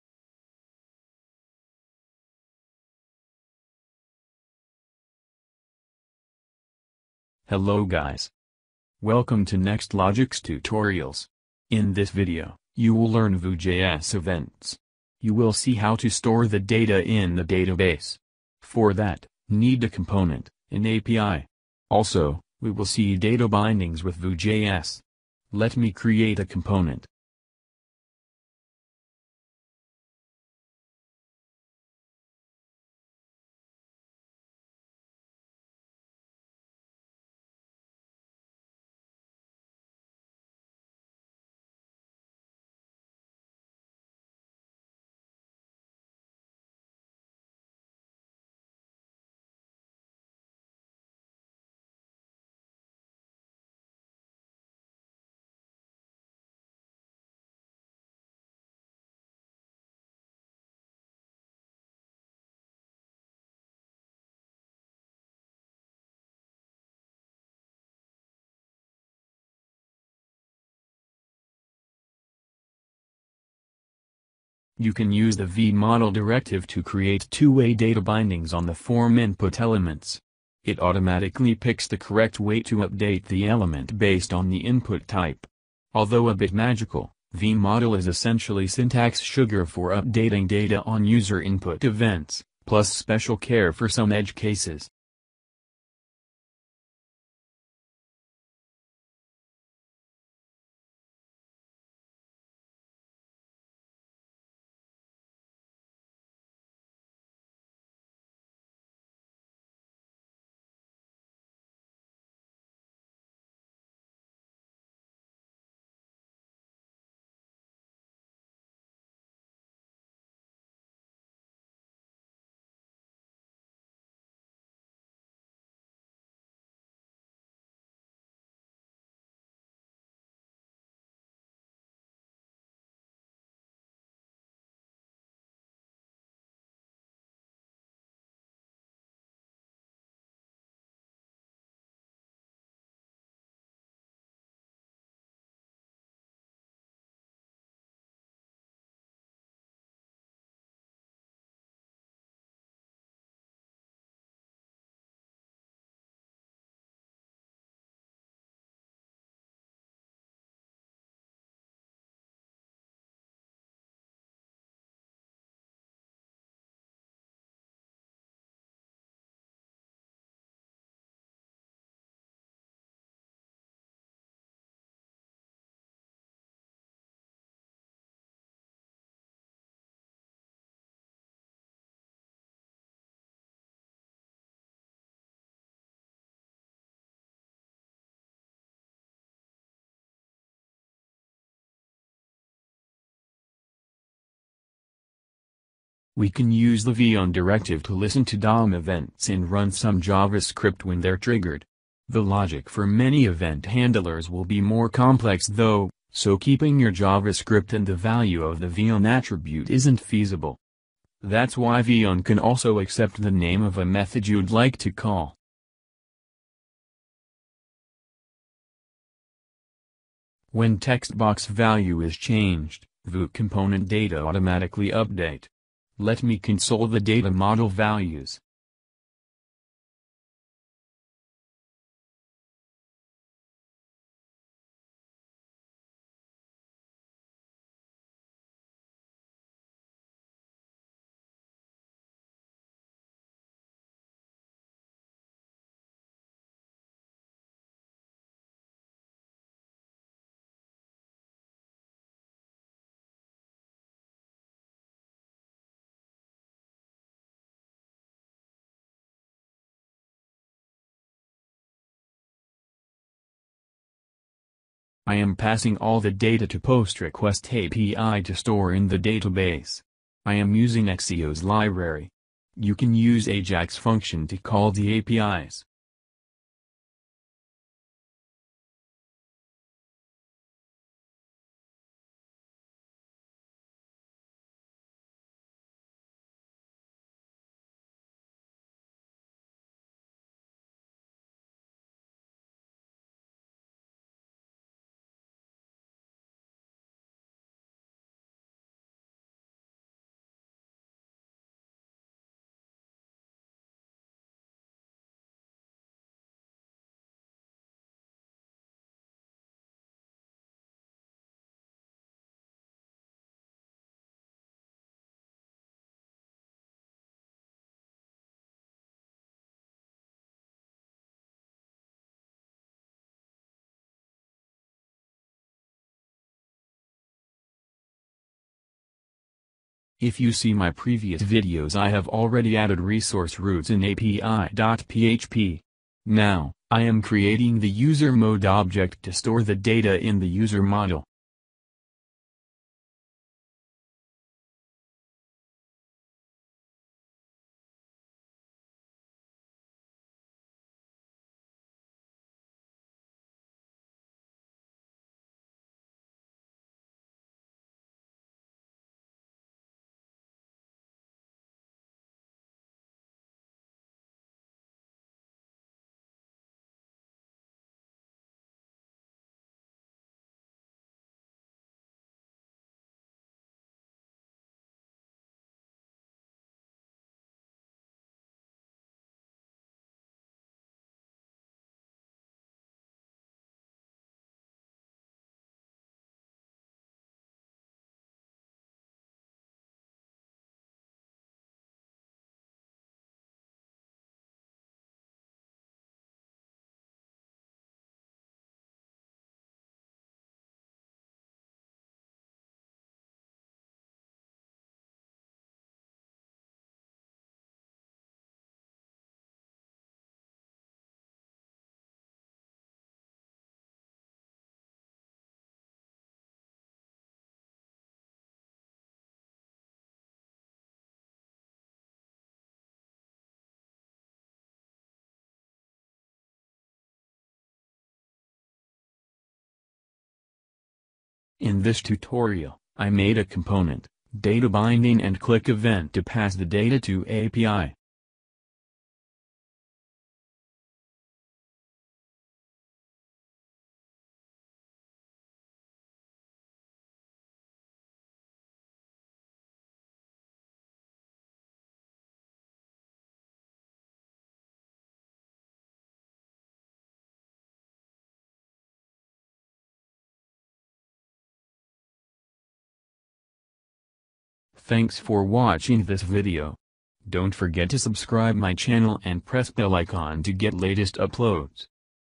Laravel 5.6 and Vue js - Data Binding and Events
In this video, you will learn two-way data binding and events.